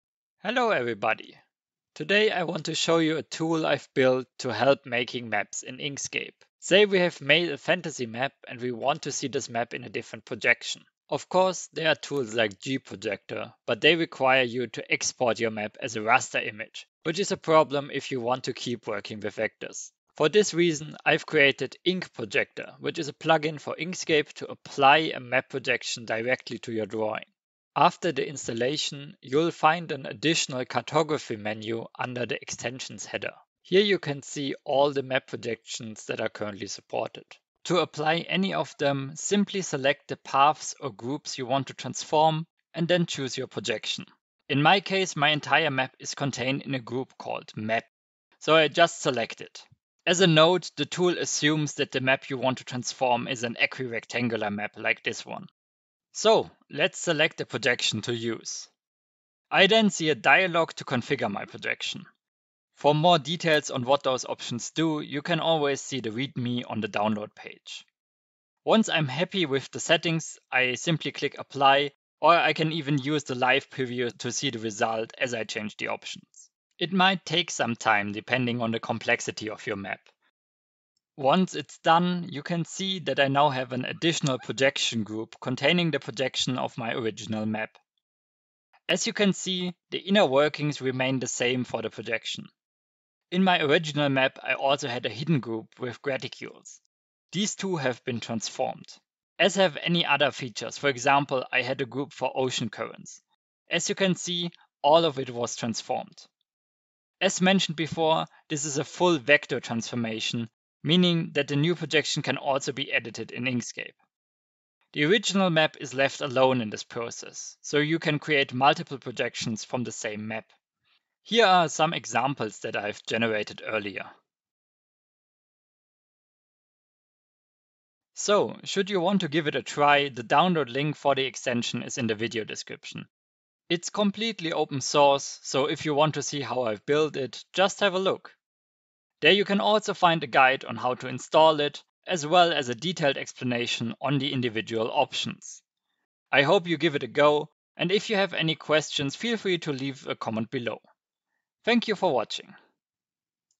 Ink Projector - Map Projections in Inkscape
A brief look at how to use the Inkscape extension Ink Projector to transform maps.